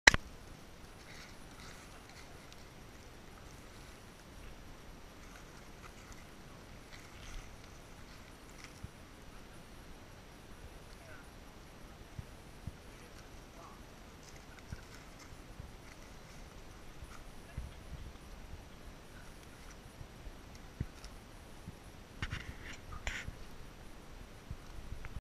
Climbing Coconut Crabs in Raja Ampat. HD.
Never thought I'd get to see tree climbing crabs like the Coconut Crab. Amazing and very creepy at the same time.
ID: IMG 0205